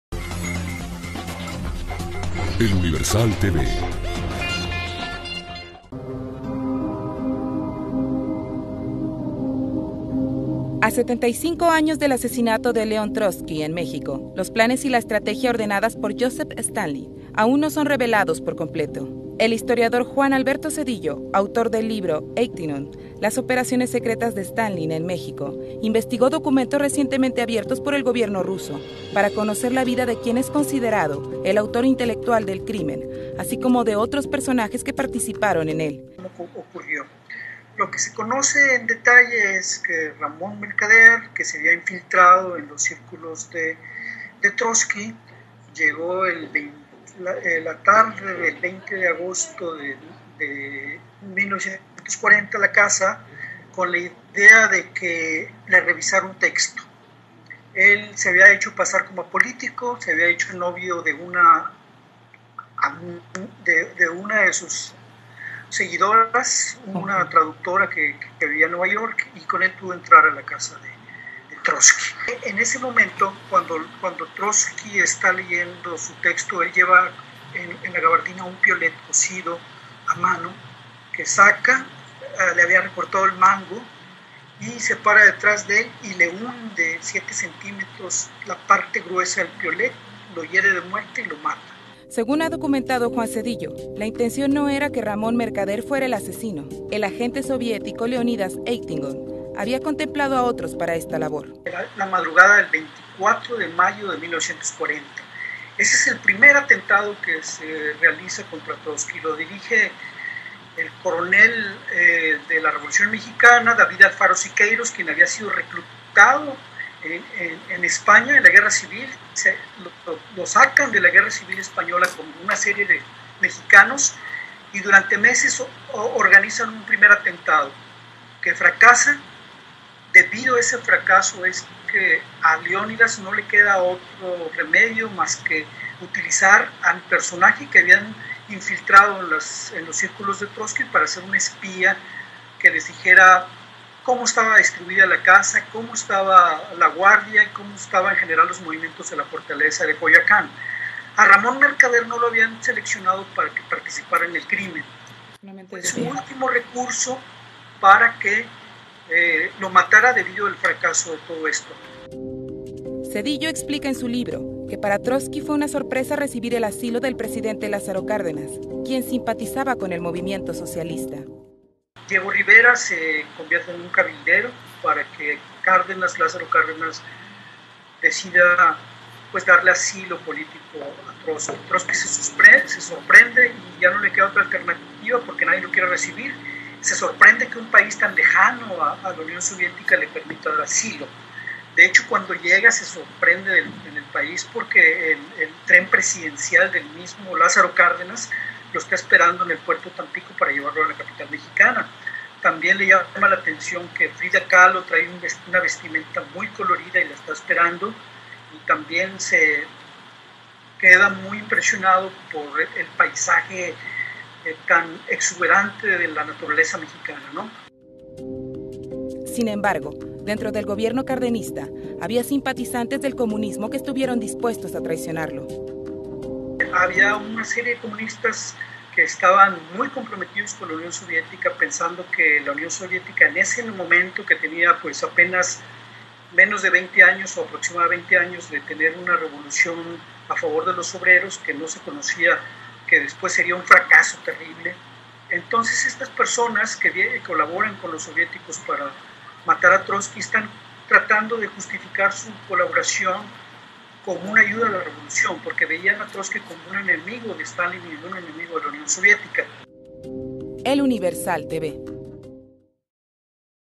El verdadero asesino intelectual de Trotsky
El historiador, Juan Cedillo, indagó en los archivos que habían permanecido secretos en Rusia, desde la desaparición de la Unión Soviética. En su libro, ´Eitingon´', las operaciones secretas de Stalin en México, revela quiénes participaron en este crimen